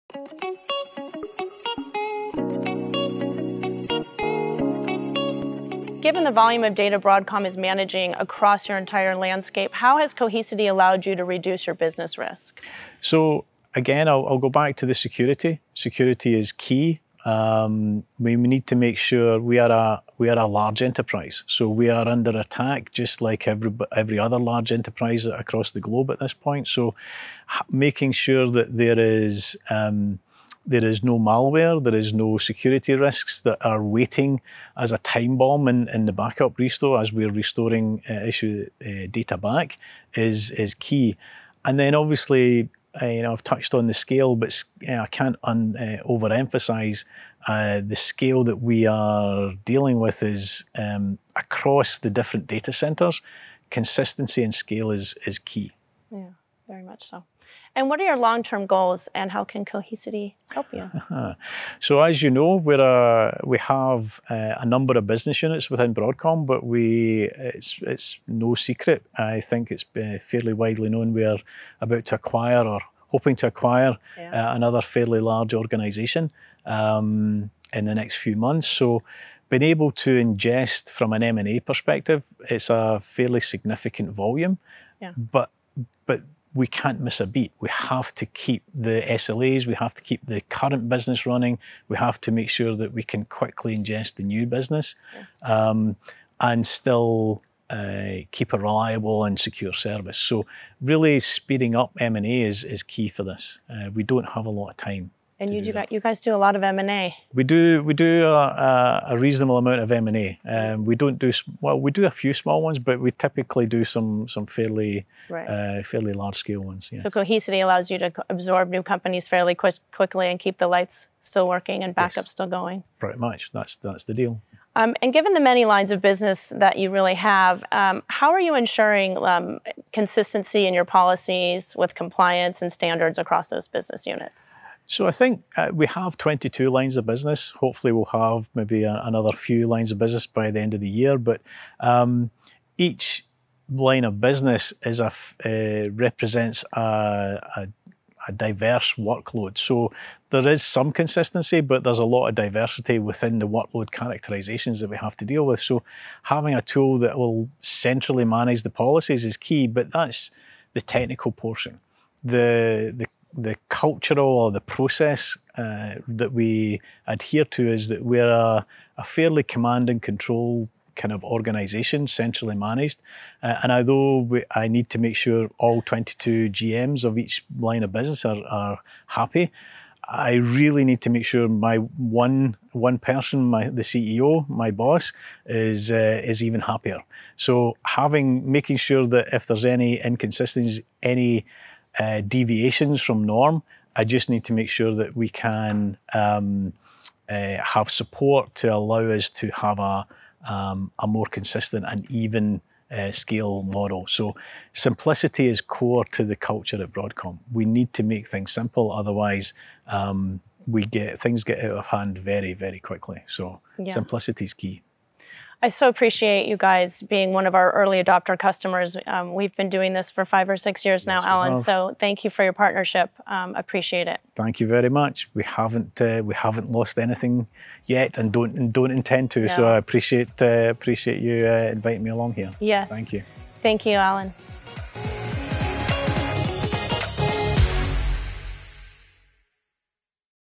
Why Broadcom chose Cohesity
During the Cohesity Catalyst conference, we sat down with Alan Davidson from Broadcom to talk about our long-standing partnership. Hear how Broadcom relies on Cohesity to secure and manage their data with a data protection and backup solution that's simple, secure, and scalable.

6400620EN102023